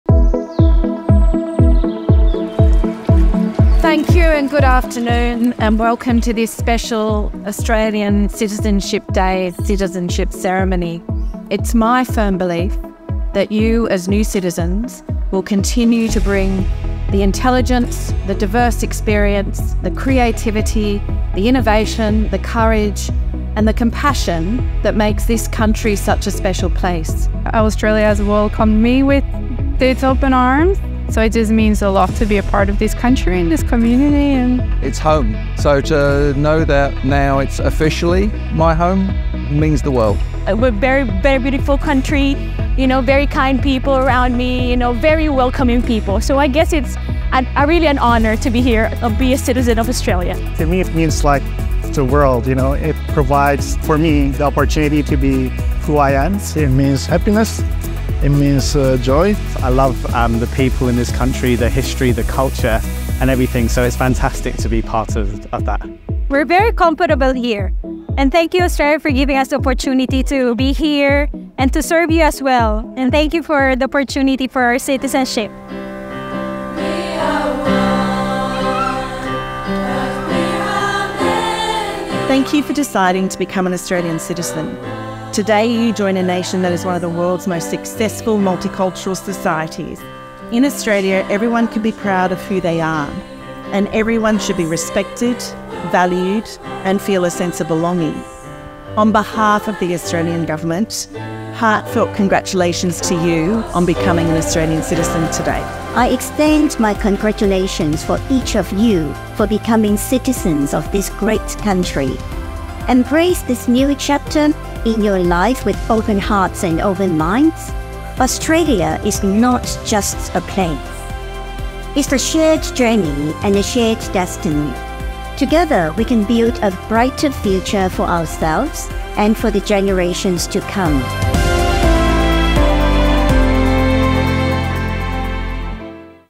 National Citizenship Day 2023
We held a special ceremony on September 15 for AustralianCitizenshipDay in Ted Mack Civic Park. Eighty North Sydney residents took the pledge to become citizens before enjoying a picnic in the park with their family and friends.